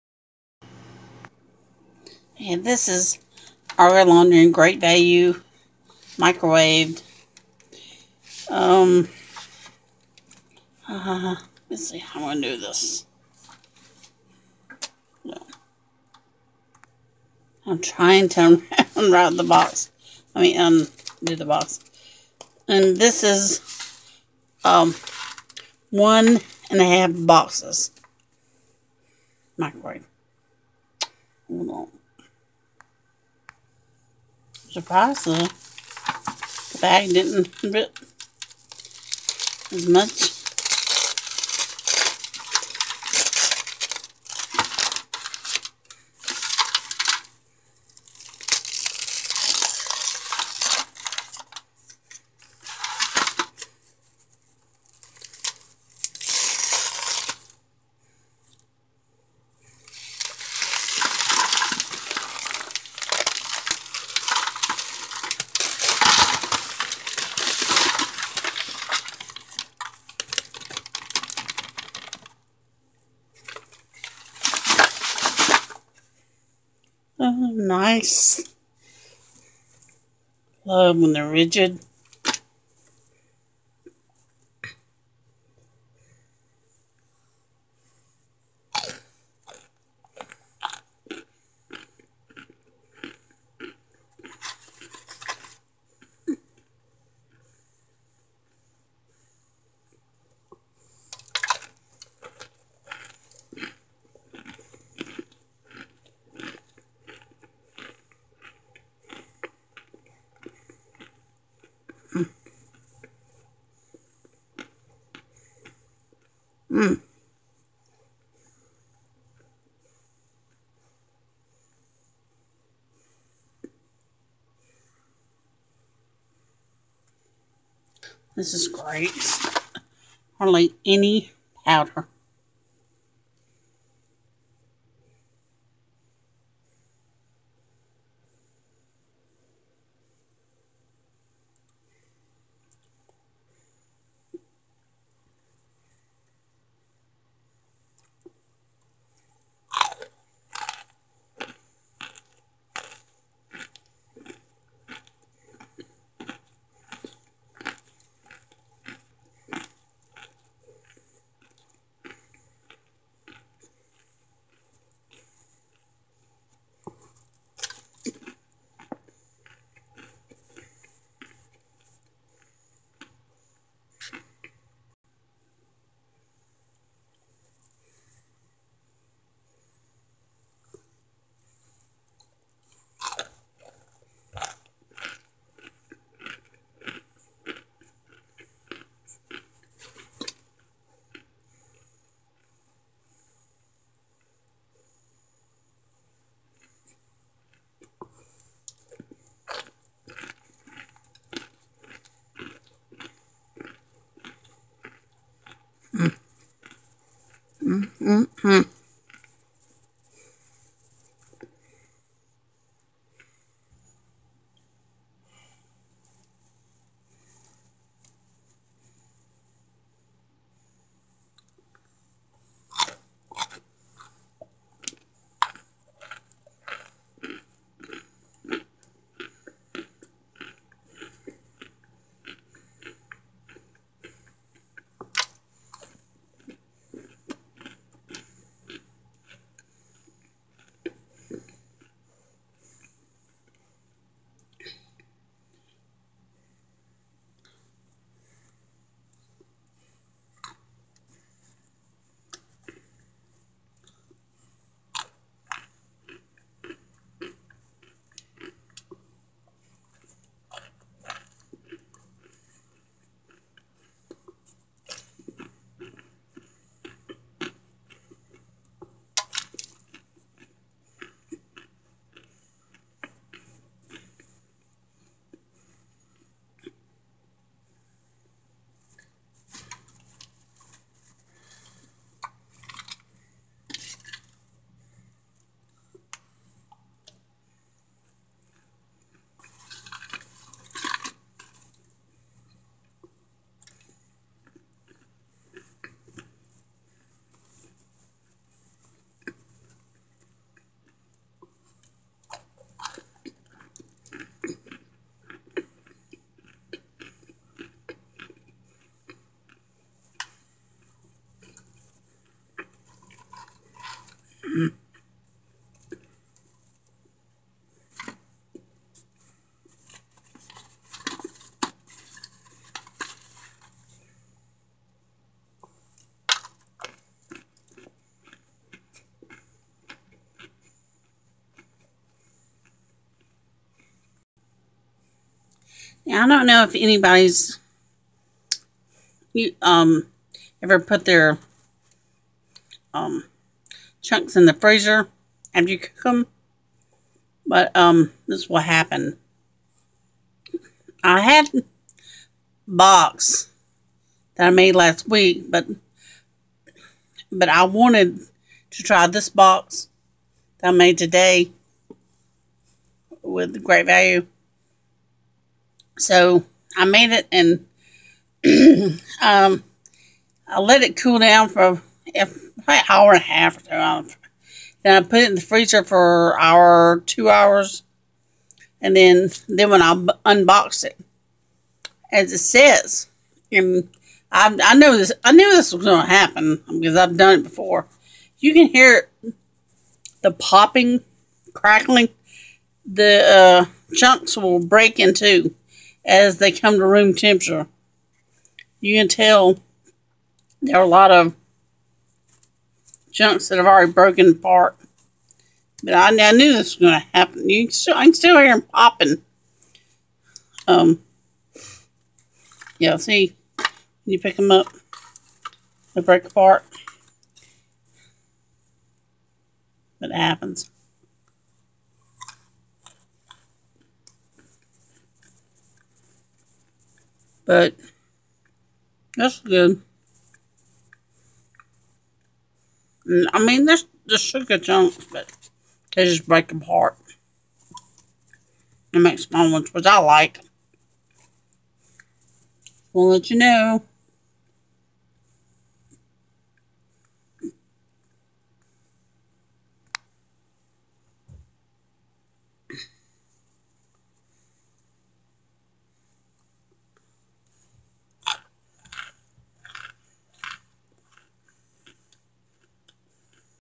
Microwave Chunks Unboxing/Snackin'(1)
Unboxing and crunching microwave chunks...ArgoL and GV
Showing why freezing chunks will break them up.
I chew only...I do not consume nor encourage anyone to consume cornstarch.